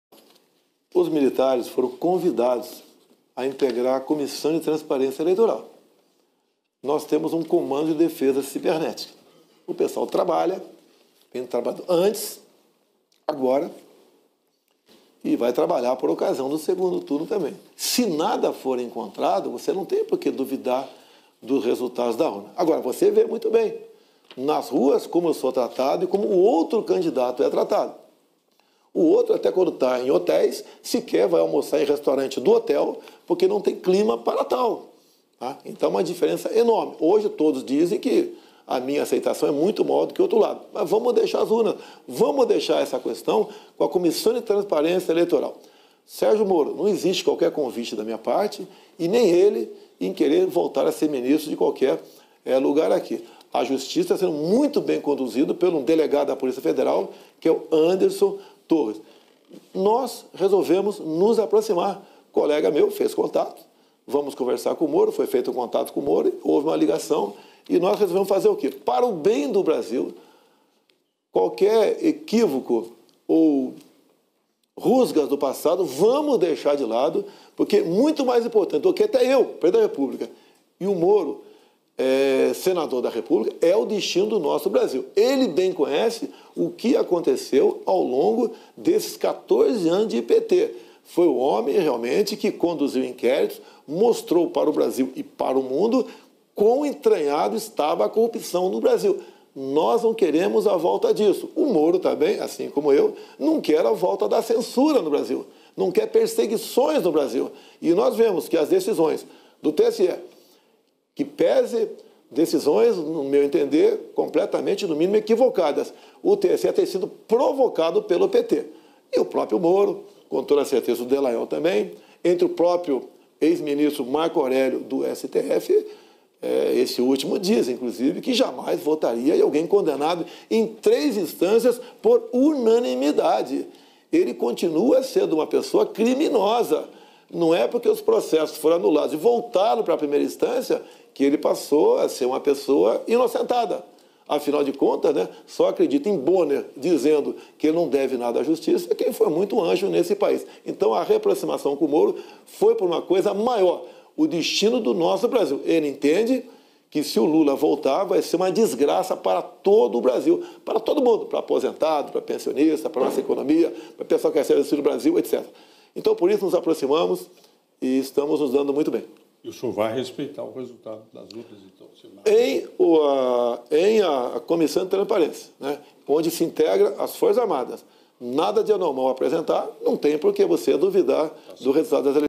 BOLSONARO SOBRE URNAS ELETRÔNICAS: "RESPEITO AO RESULTADO DEPENDE DE RELATÓRIO DAS FORÇAS ARMADAS"
Última pergunta do último bloco da entrevista com Jair Bolsonaro (PL). O presidente responde se, se derrotado, acatará o resultado das urnas eletrônicas. Veja o que ele disse!